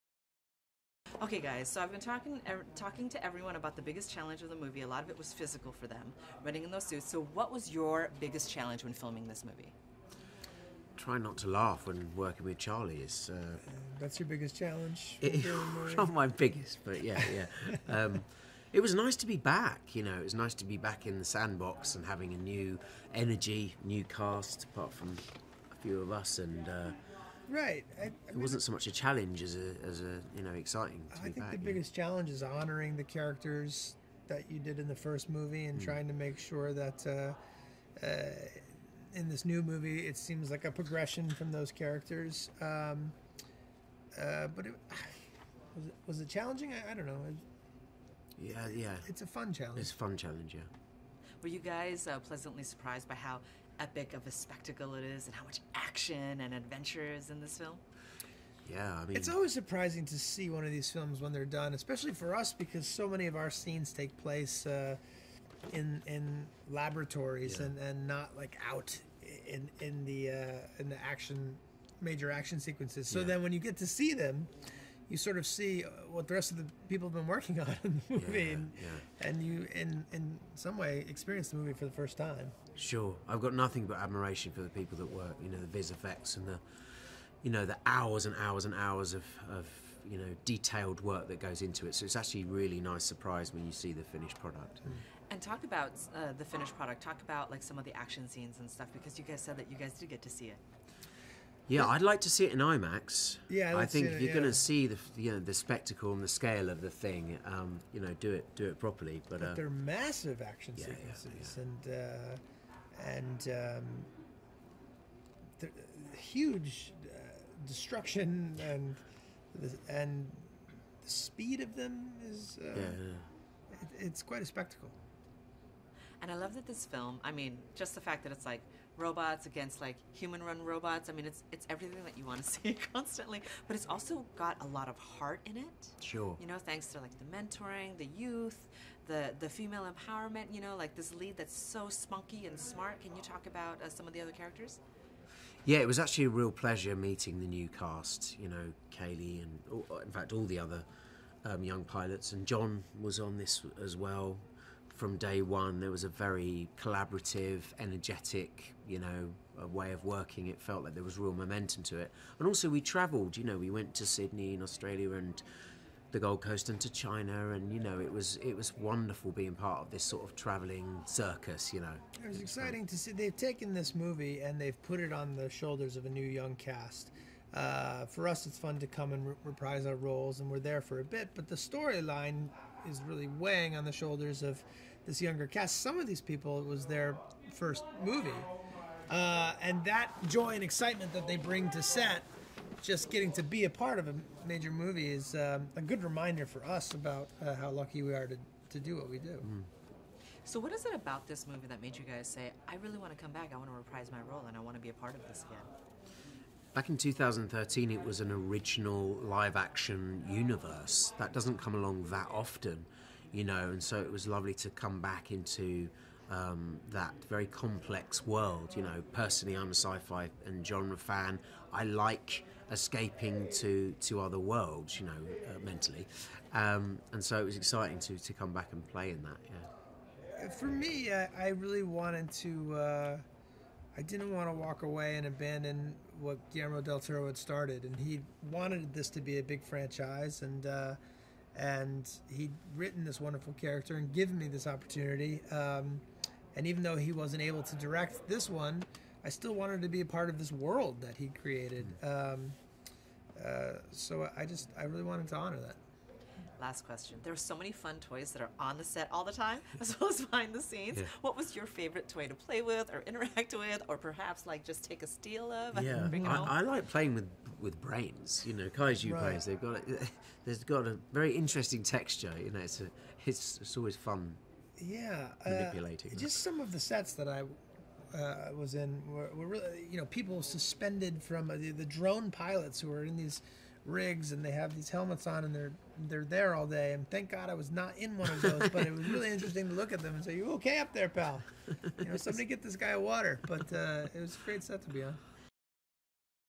Pacific Rim Uprising - Itw Burn Gorman Charlie Day (CamA) (Official video)
Pacific Rim Uprising is an upcoming American science fiction action film written and directed by Steven S. DeKnight in his feature film directorial debut and co-written by Emily Carmichael, Kira Snyder, and T.S. Nowlin. It is the sequel to the 2013 film Pacific Rim by Guillermo del Toro. It stars John Boyega, Scott Eastwood, Cailee Spaeny, Jing Tian, and Adria Arjona, with Charlie Day, Burn Gorman, and Rinko Kikuchi returning in their roles from the original film. It is scheduled to be released on March 23, 2018, by Universal Pictures worldwide. The film will be released in 2D, Real D 3D, IMAX 3D, and IMAX release.

Synopsis:
Ten years after the Battle of the Breach, the oceans have become restless once again, but the Jaeger program has evolved into the next generation for the PPDC. However, something has reopened the Breach for the Kaiju and a Jaeger has gone rogue. Jake Pentecost, son of Stacker Pentecost, rises up to stand against the evolved Kaiju and the rogue Jaeger to prevent humanity's extinction and preserve his father’s legacy.

Pacific Rim Uprising
Directed by Steven S. DeKnight
Produced by John Boyega, Cale Boyter, Guillermo del Toro, Jon Jashni, Femi Oguns, Mary Parent, Thomas Tull
Screenplay by Emily Carmichael, Kira Snyder, Steven S. DeKnight, T.S. Nowlin
Story by Steven S. DeKnight, T.S. Nowlin
Based on Characters by Travis Beacham
Starring John Boyega, Scott Eastwood, Cailee Spaeny, Charlie Day, Burn Gorman, Jing Tian, Adria Arjona, Rinko Kikuchi
Cinematography : Dan Mindel
Edited by Dylan Highsmith, Zach Staenberg
Production company : Legendary Pictures, UpperRoom Entertainment Limited, DDY
Release date : March 23, 2018
Running time : 98 minutes

(Source: Wikipedia)